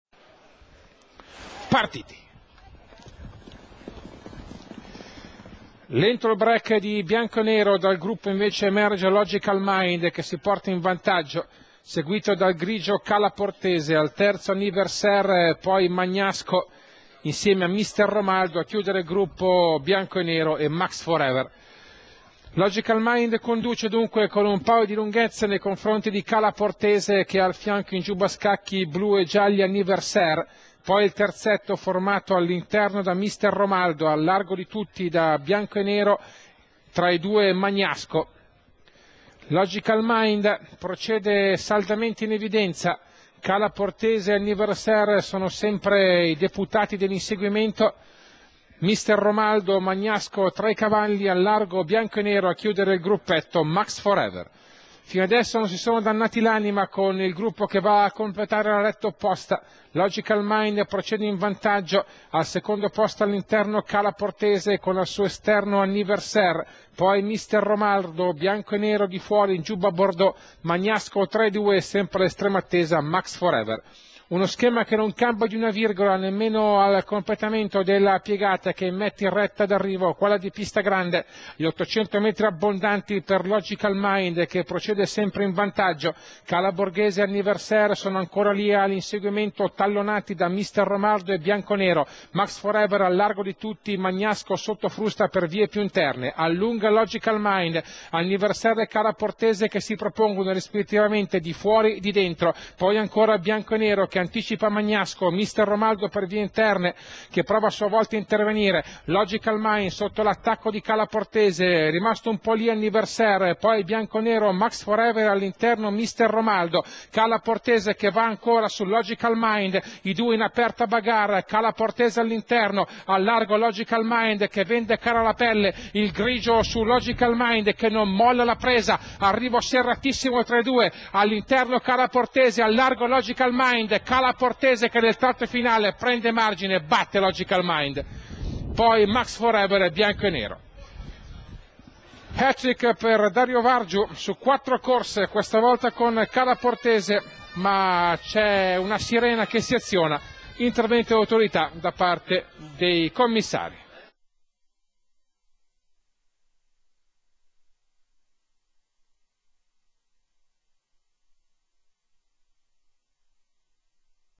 07/04/2013, Milano , Premio Ticino
1. LOGICAL MIND (FRESU ANT.), 2. CALA PORTESE (VARGIU DARIO), 3. MAX FOREVER (MANIEZZI LUCA), 4. BIANCO E NERO (COLOMBI CLAUDIO), 5. MAGNASCO (BOSSA FEDERICO), 6. MISTER ROMALDO (CONVERTINO P.), 7. ANNIVERSAIRE (BRANCA FABIO)